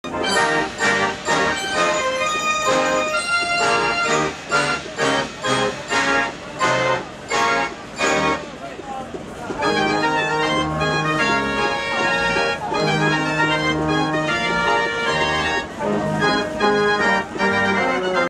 General Gough Pickering 2008
General Gough on Saturday Night at Pickering steam rally 2008